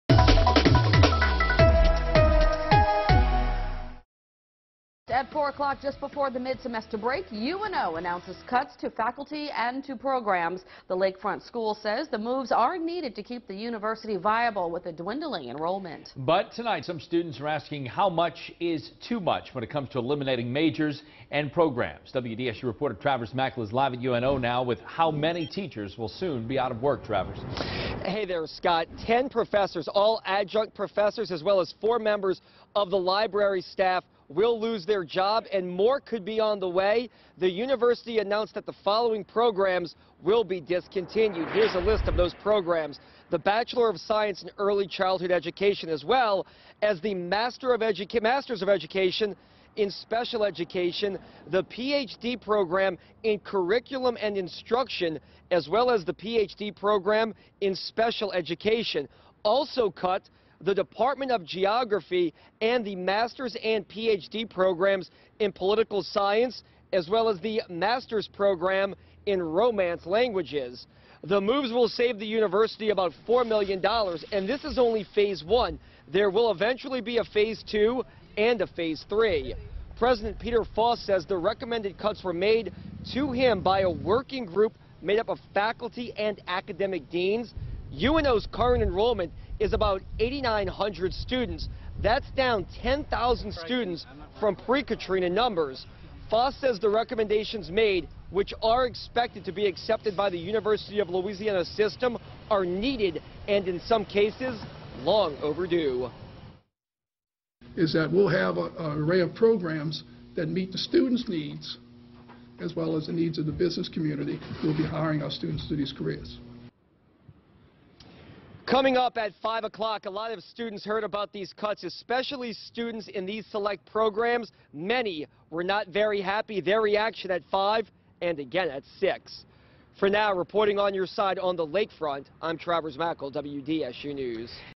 UNO announces more cuts in restructuring plan
The University of New Orleans announced major cuts to academic programs in a restructuring plan.